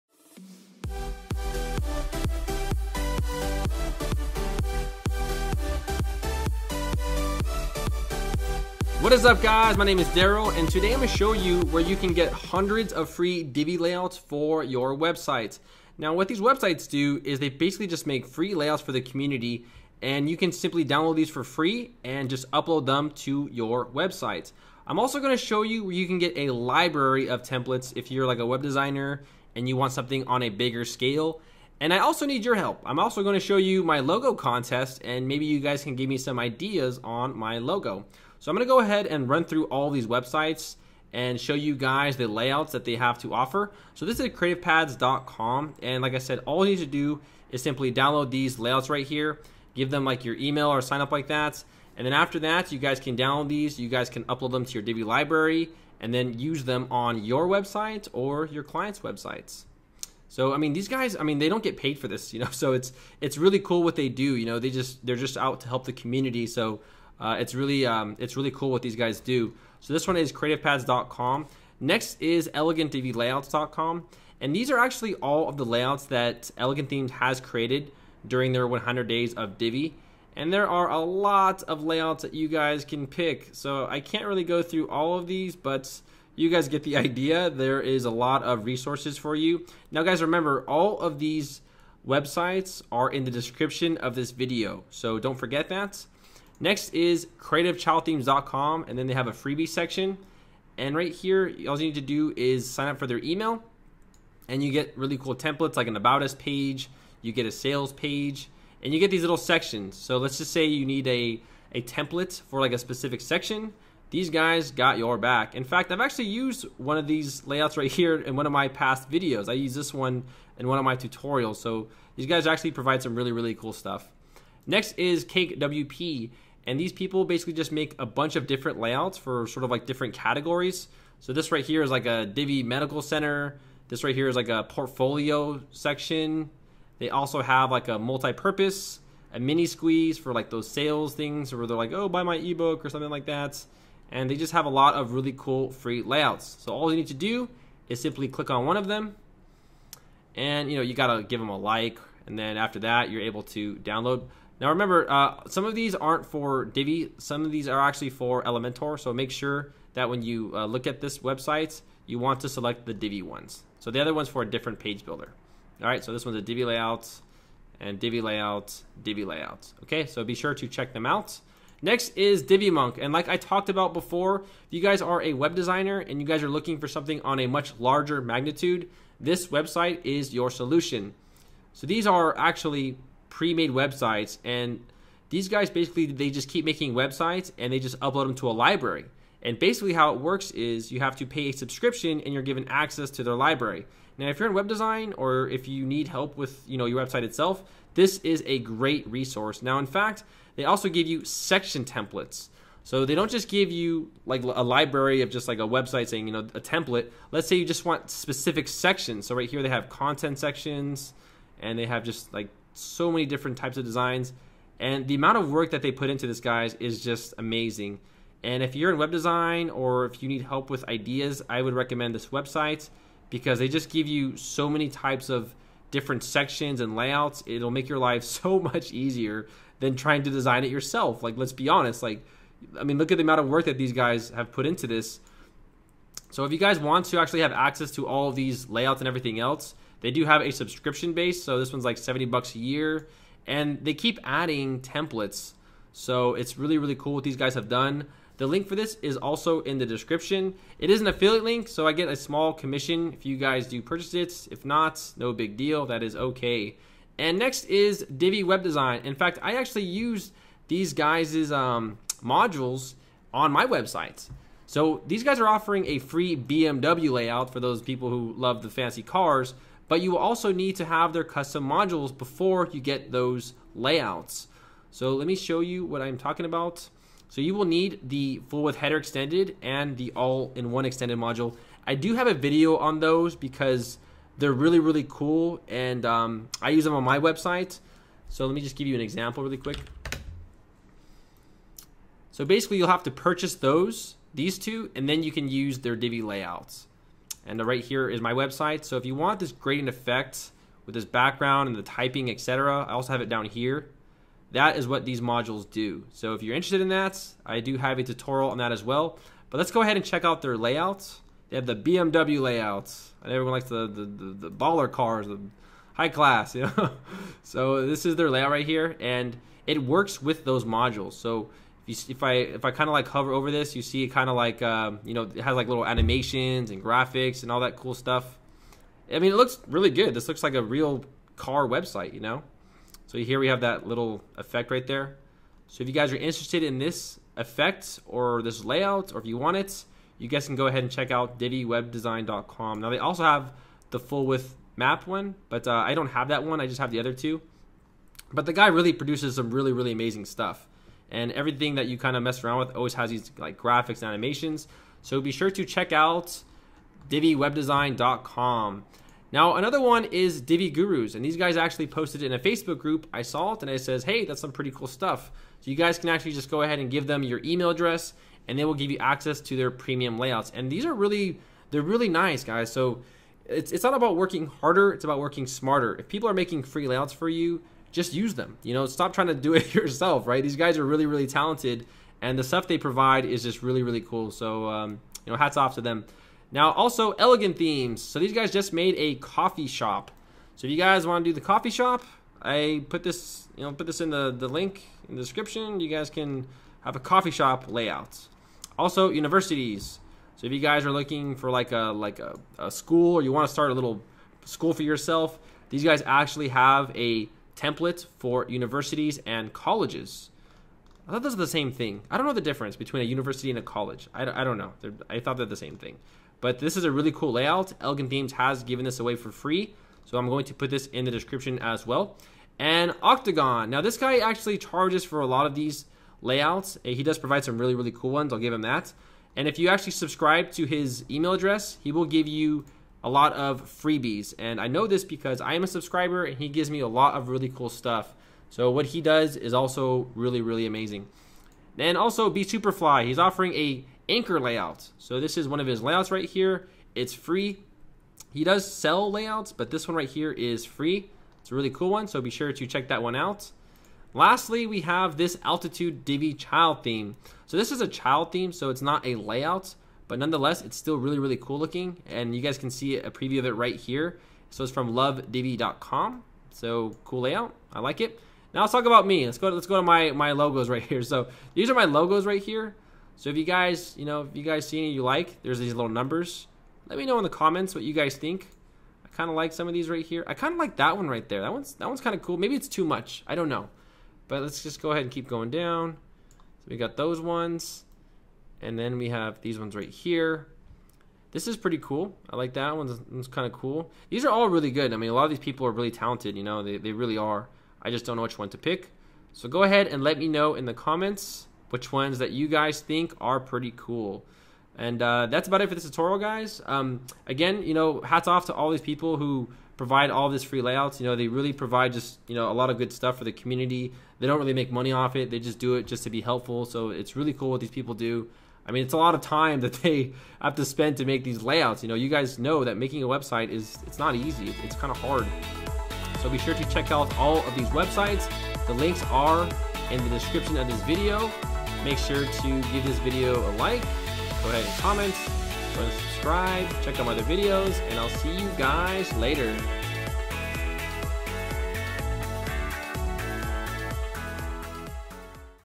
FREE+NEW Divi Theme 3.0 Layouts For Wordpress 2017 ✅ 😀
Welcome! Do you want some free divi layouts and good resources  for the divi theme? In this video, i am going to show you where you can get free divi layouts for your wordpress website

I hope these divi theme 3.0 layouts help you with your website. I have tons of divi theme tutorials so be sure to check them out!

For more tutorials like this or to learn how to make a wordpress website, feel free to visit my